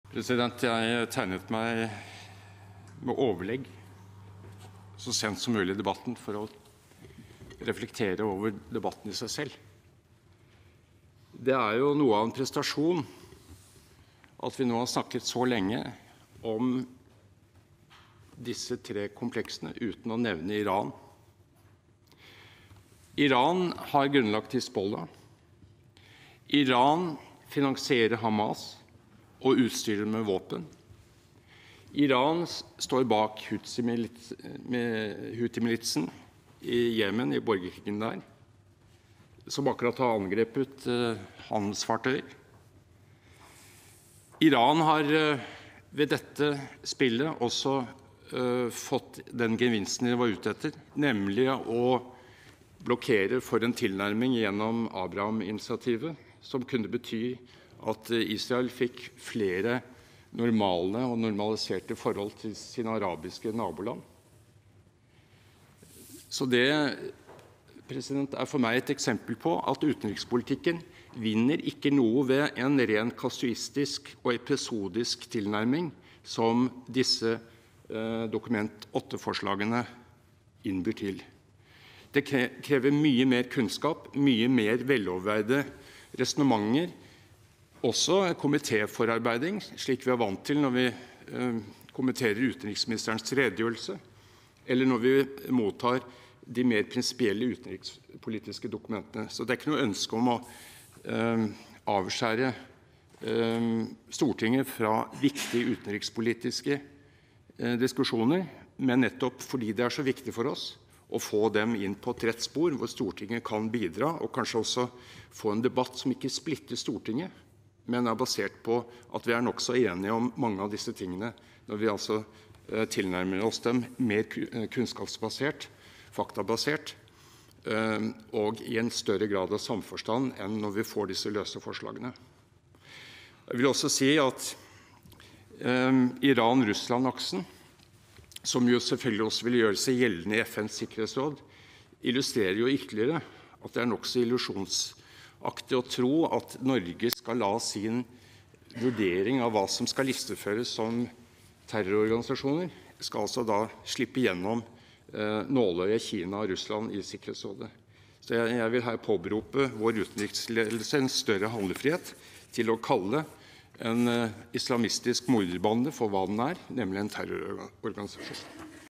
05.12.2023 Sak 4 6 Michael Tetzschner
Om humanitær innsats for å evakuere skadde barn på Gazastripen og tilby dem helsehjelp (Innst. 116 S (2023–2024), jf. Dokument 8:43 S (2023–2024)); Om å fryse bistand til palestinske selvstyremyndigheter (Innst. 49 S (2023–2024), jf. Dokument 8:15 S (2023–2024)) og om å liste Hamas som terrororganisasjon (Innst. 43 S (2023–2024), jf. Dokument 8:16 S (2023–2024))